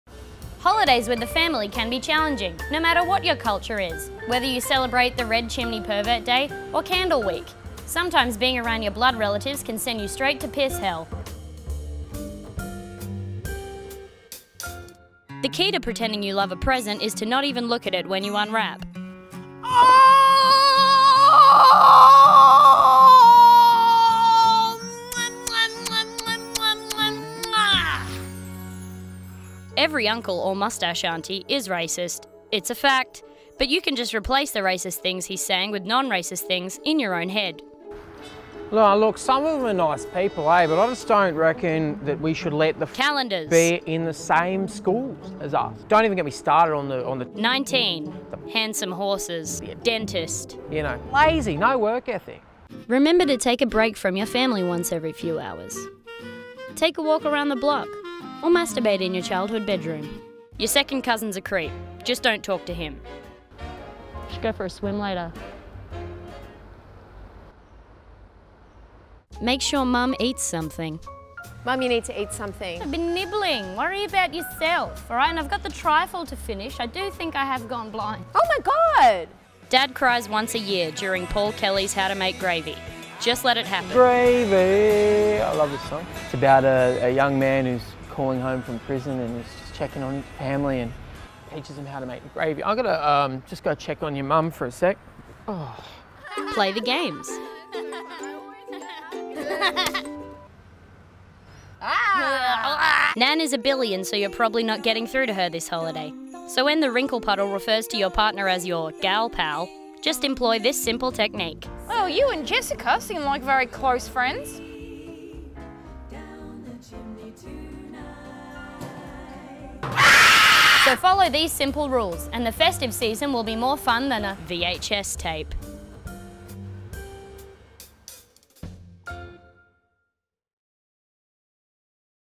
How to survive the festive season
Christmas is a time of giving, joy, and pretending to like some of your relatives.